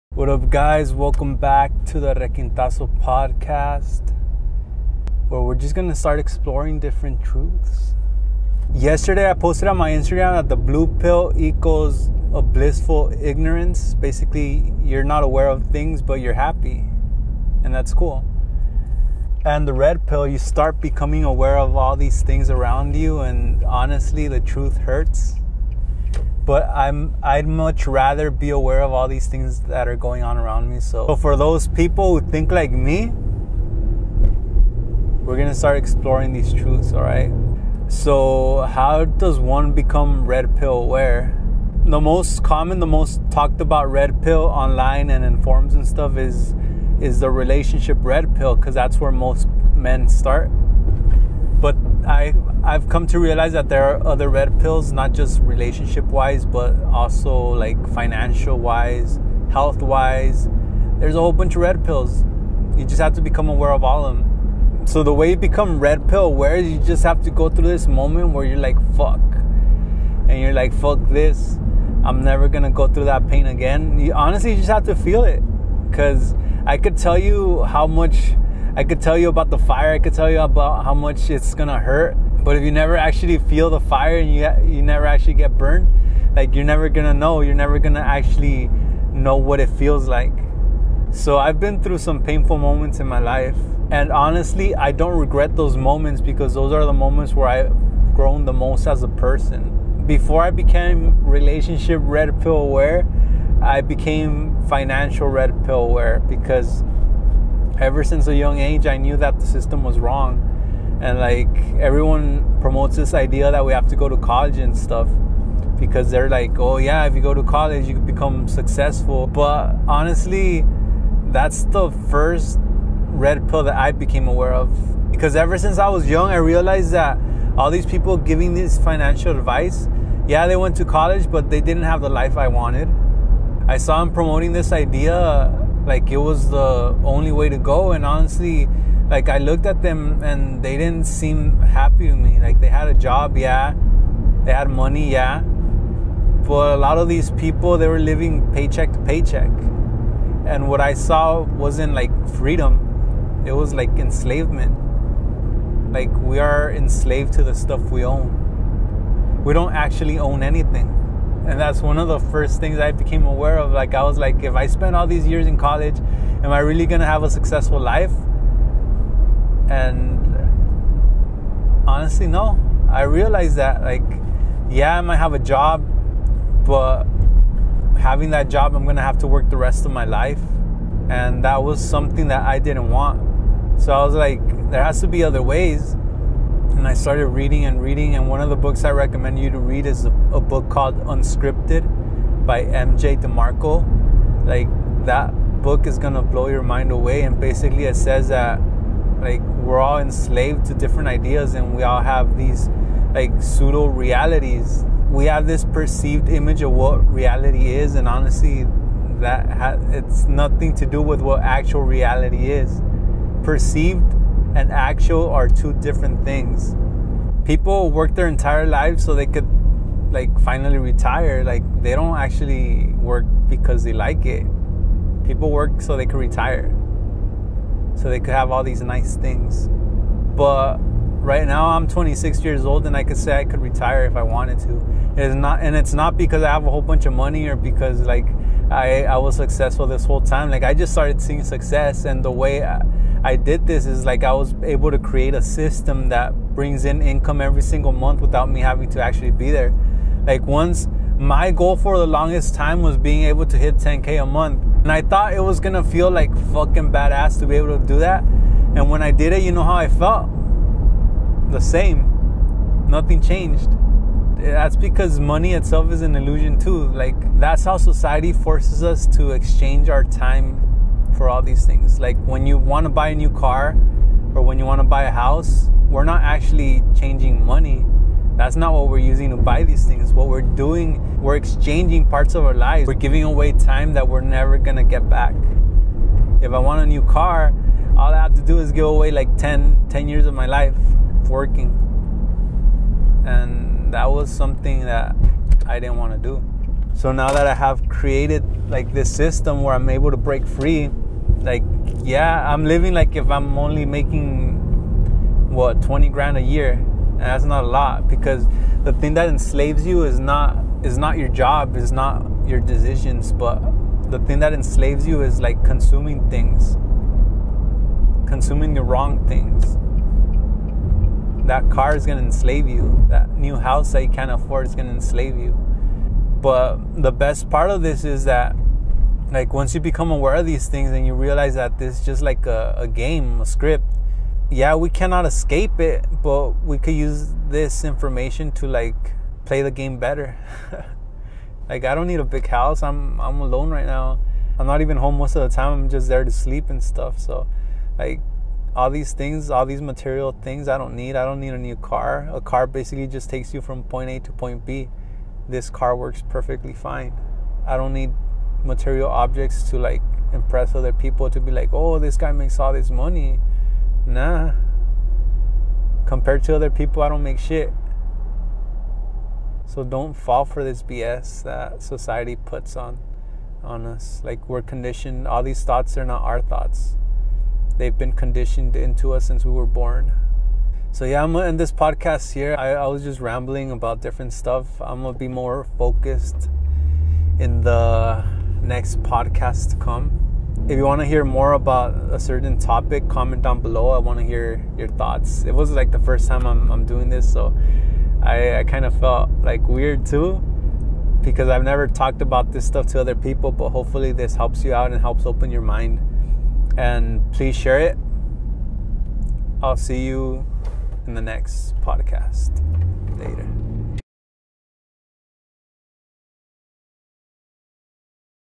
Are there other Red Pills?? | Requintazo Podcast
Red pill equals unpleasant truth. the most commonly talked about red pill is the one on relationships. However, as I become more aware of things I came to realize that there were other Red pills. Here are my thoughts on jobs and how we are forced into slavery without us knowing it.

-Jovanny Castillo
Requintazo.com